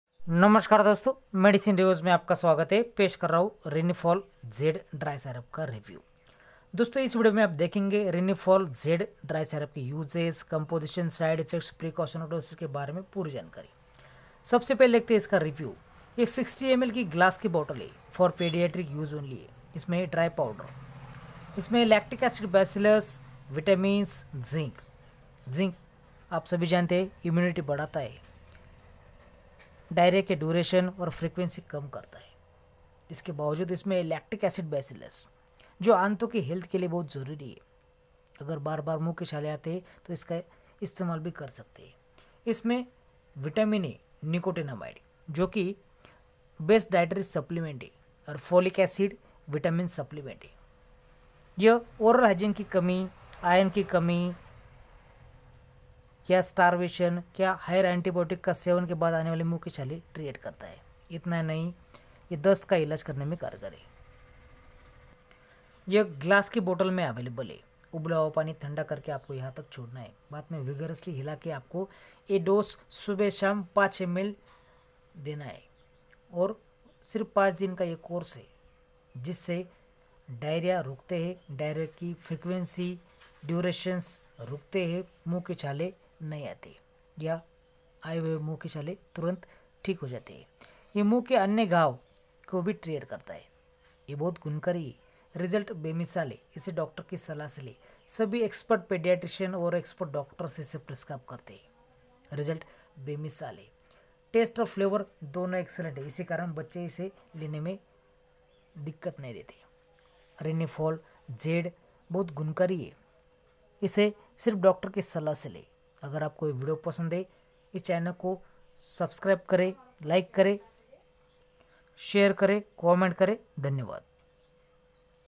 Rinifol Z Dry Syrup review in Hindi बच्‍चों को डायरिया व दस्‍त, मुंह के छाले होने पर अपनाएं यह दवा !
Rinifol Z Dry Syrup is used as supplement and used in the treatment of indigestion. Rinifol Z Dry Syrup contains Lactobacillus, Vitamin B3 (Niacinamide), Folic Acid, Vitamin B6(Pyridoxine) and Zinc.
Role of key ingredients:
Lactobacillus Sporogenes is a probiotic which supports the growth of friendly bacteria and helps maintain a healthy balance of microflora in the intestinal environment.
Vitamin B3 (Niacinamide), Folic Acid, Vitamin B6(Pyridoxine) shows excellent antioxidant properties.Vitamin B Complex is an essential nutrient that converts the food into energy. It also helps the body make healthy new cells and boost good cholesterol level. It works as an antioxidant, which is necessary for transporting oxygen throughout the body.
Zinc supports the immune function and further promote the intestinal health.

Rinifol Z Dry Syrup is used in indigestion, abnormal distension, improper metabolism, loss of appetite, nutritional aid during convalescence and all types of digestive disorders.
Use under medical supervision online-medicine , medicine,Medicine Reviews Instagram id vijaylahore4262